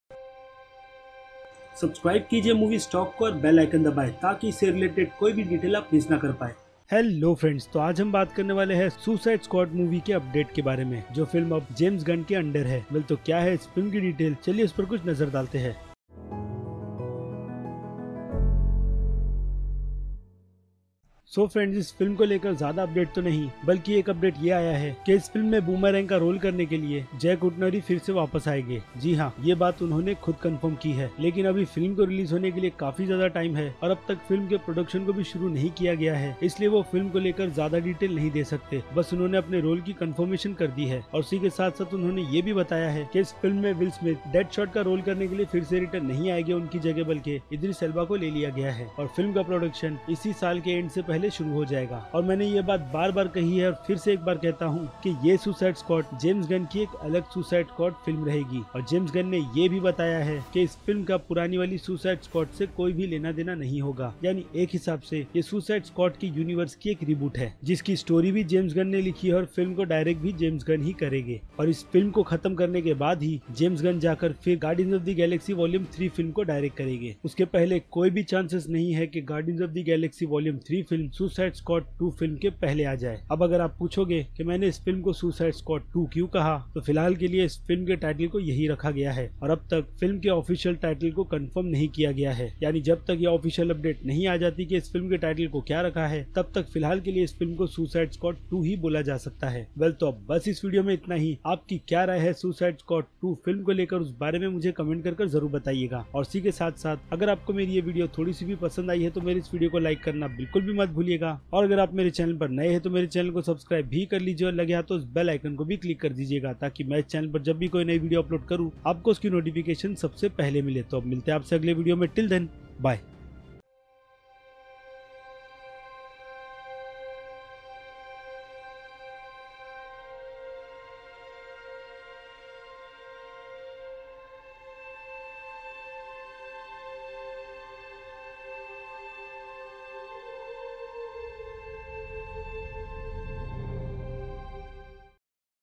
Captain Boomerang Is Back For Suicide Squad 2 Film [Explained In Hindi]
Hello friends aaj hum baat karne wale hai marvel's Suicide Squad 2 film ke bare ke me to kya hai woh chlaiye dekhte hai